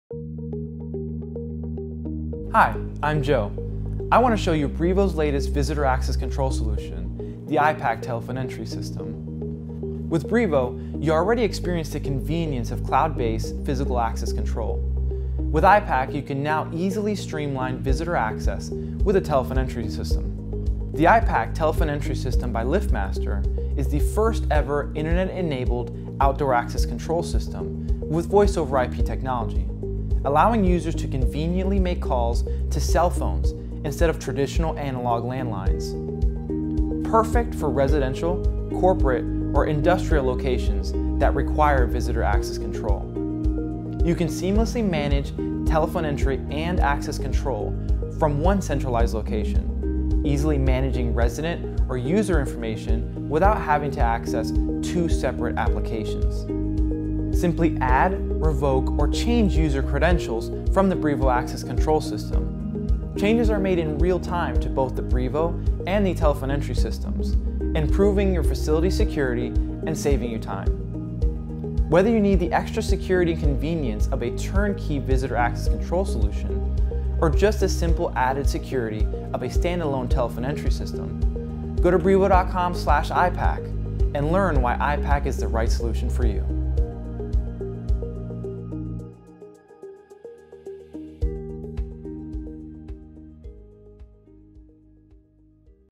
LiftMaster - IPAC Telephone Entry System and Brivo Software
Discover all of the advanced features of LiftMaster's internet enabled, cloud based IPAC has to offer.